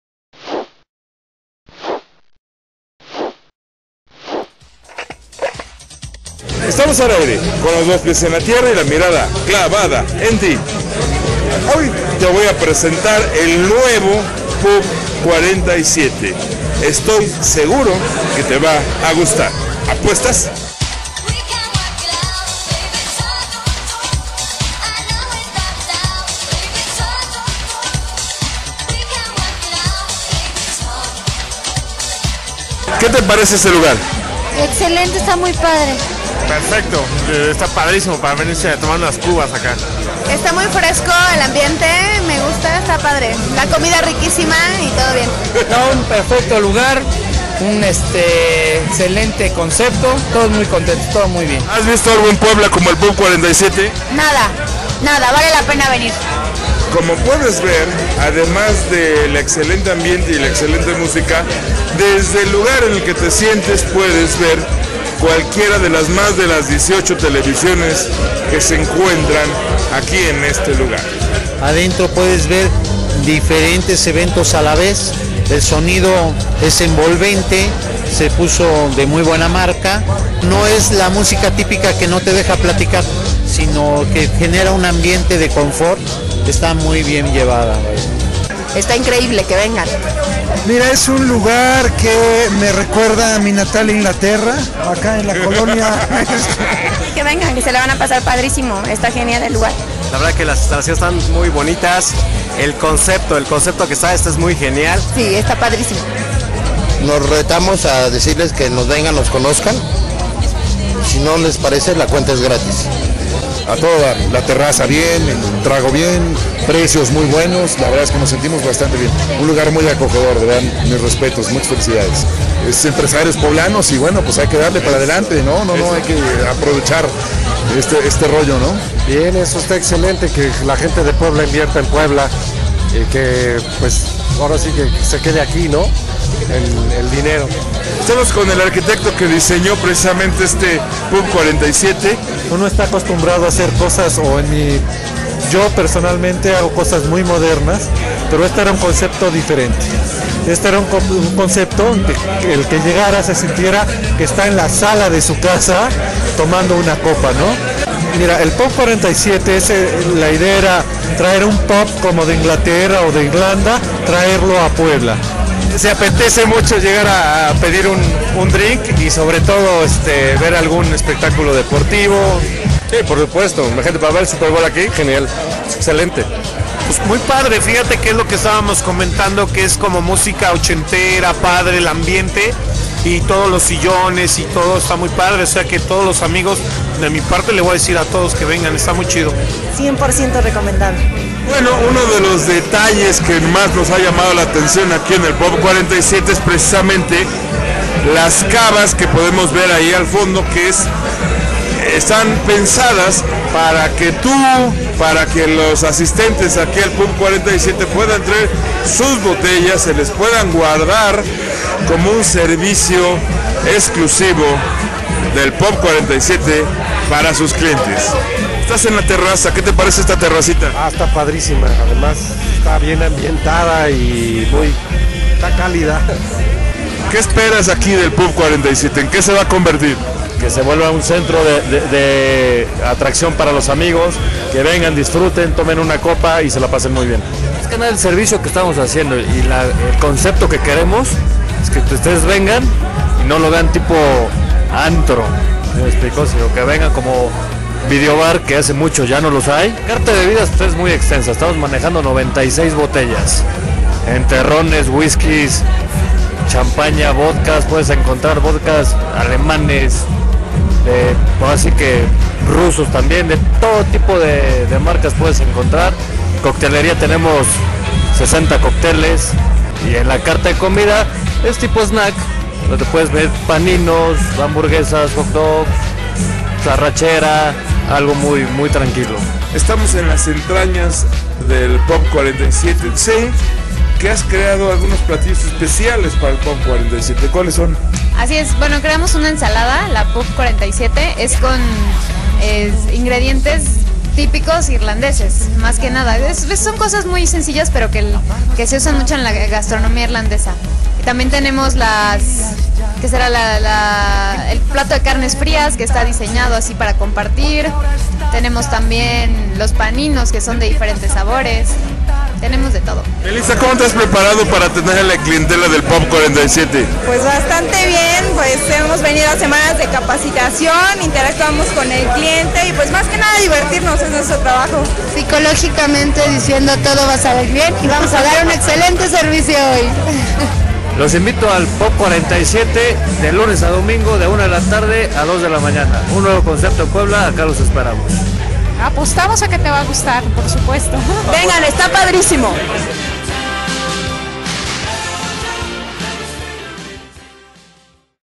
Pub 47 ► Empresas en la Jungla de Mariano
Conoce el Pub 47. Una inversión poblana para crear un ambiente ideal para fomentar amistades, disfrutar de buena música, bebida y comida siguiendo tus deportes favoritos. Apostamos que te va a gustar.